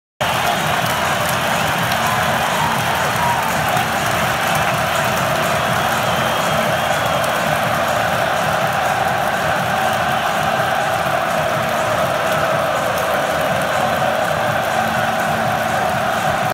Swansea City give Manchester City a guard of honour after winning Premier League title
Got something we should upload let us know!